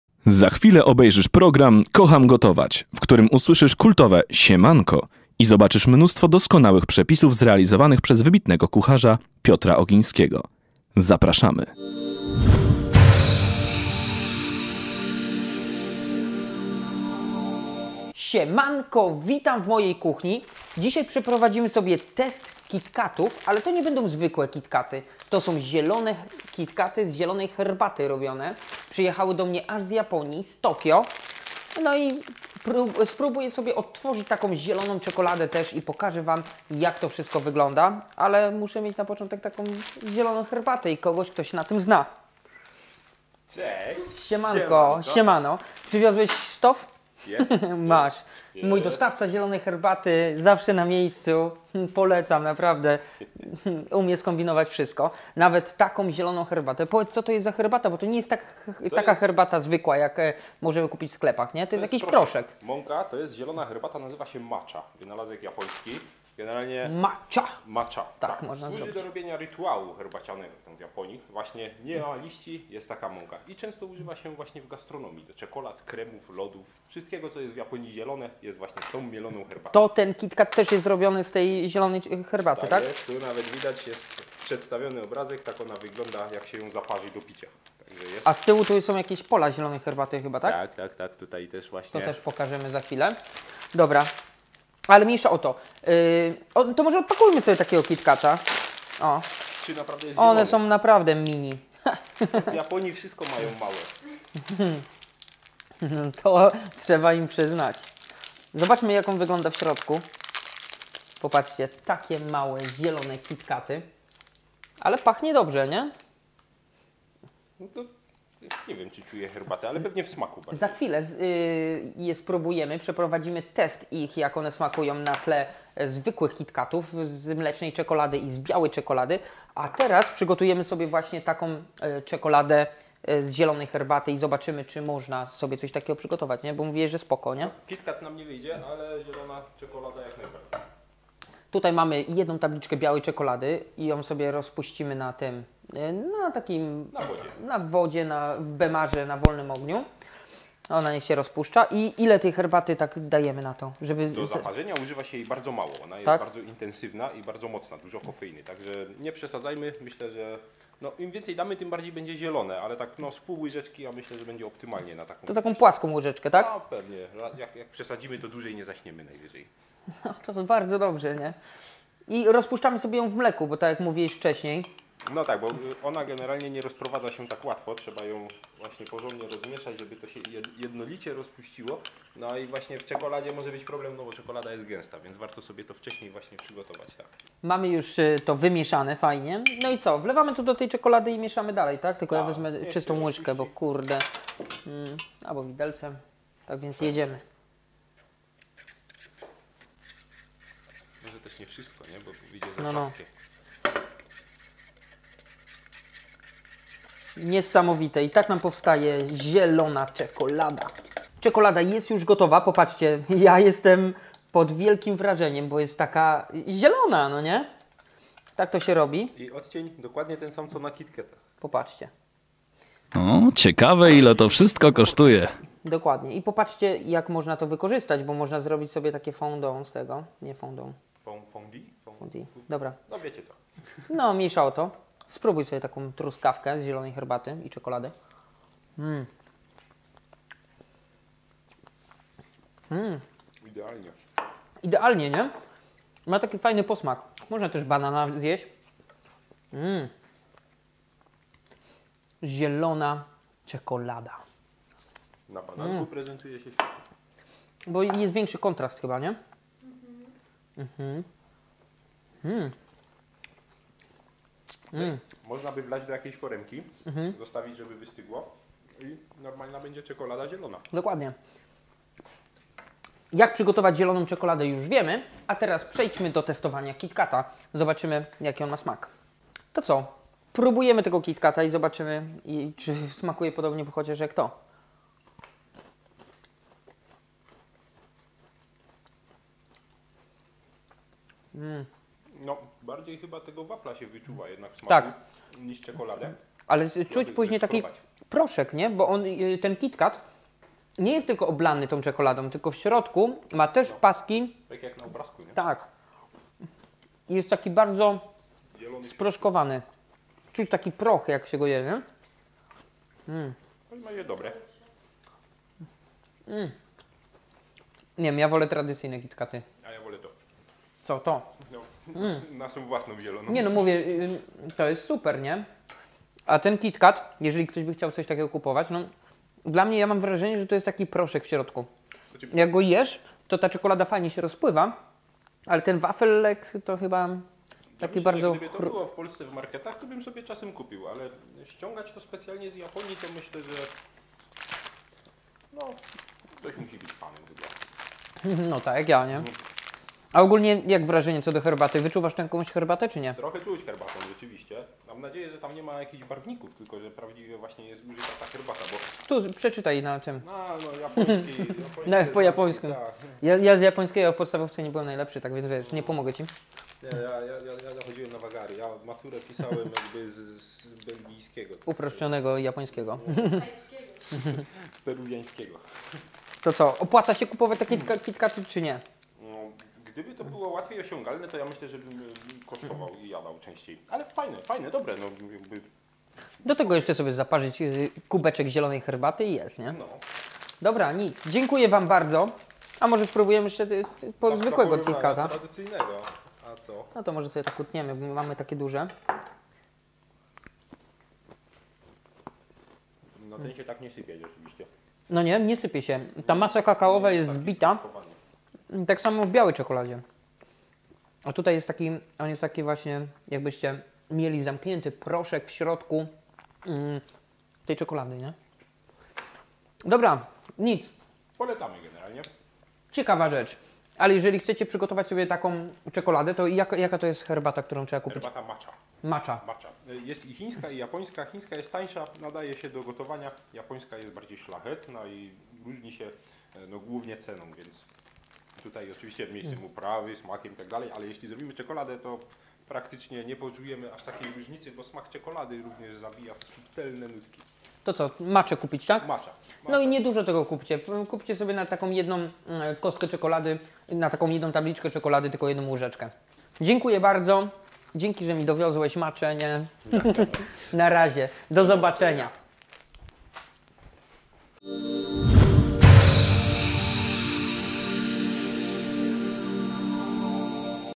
TEST zielonych KiT KaTów キットカット 大人の甘さ 抹茶
Zieloną herbatę  Matcha  抹茶, dostarczył mi Rafał z czajnikowy.pl
Znajdziecie go tutaj